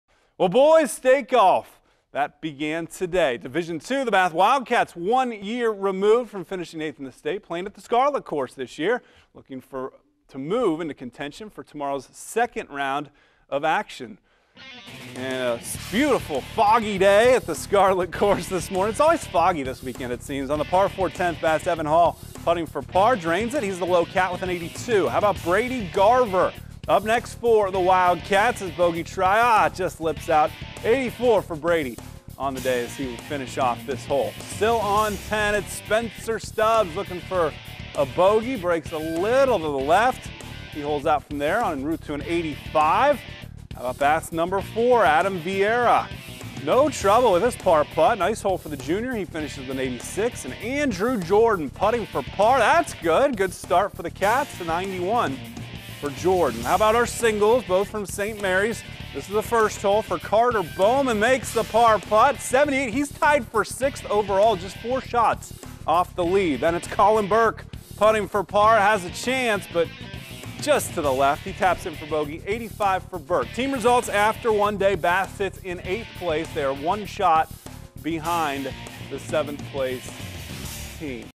D2 State Golf from Scarlett Course - Day 1
Bath Golf competes at state along with two individual competitors from St. Marys